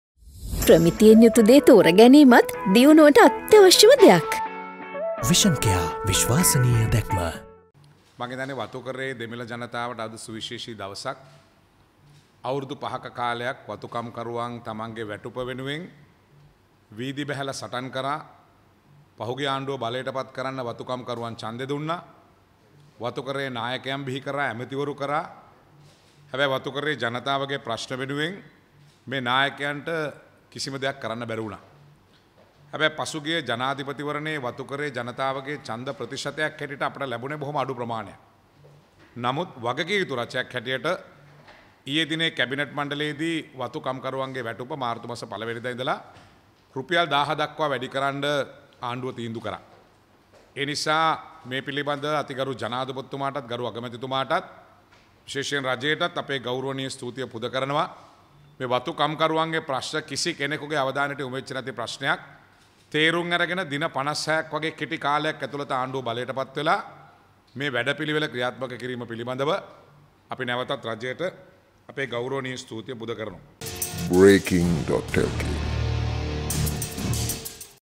මාර්තු පළමුවෙනිදා සිට වතු කම්කරුවන්ගේ වැටුප රුපියල් 1000කින් ඉහළට | Breaking.lk
මාර්තු පළමුවෙනිදා සිට වතු කම්කරුවන්ගේ වැටුප රුපියල් 1000කින් ඉහළට